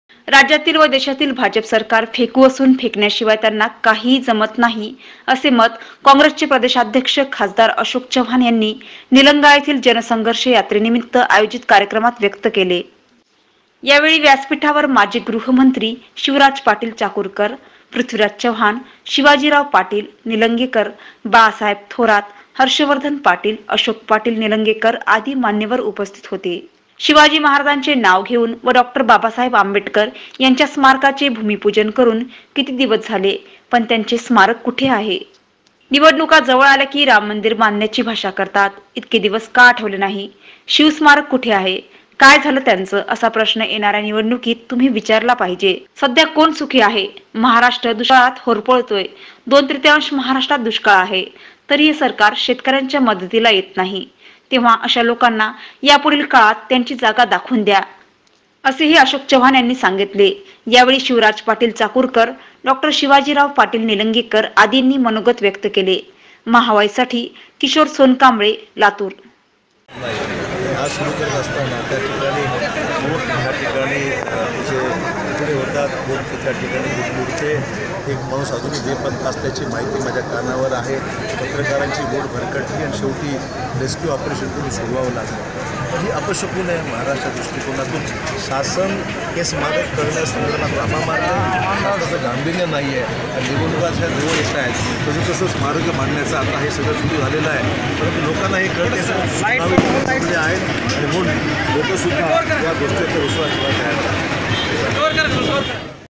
The current government False... MP.Ashok Chavan
लातूर -राज्यातील व देशातील भाजप सरकार फेकू असून फेकण्या शिवाय त्यांना काहीही जमत नाही असे मत काँग्रेस चे प्रदेशाध्यक्ष खा. अशोक चव्हाण यांनी निलंगा येथील जन संघर्ष यात्रे निमित्त आयोजित कार्यक्रमात व्यक्त केले.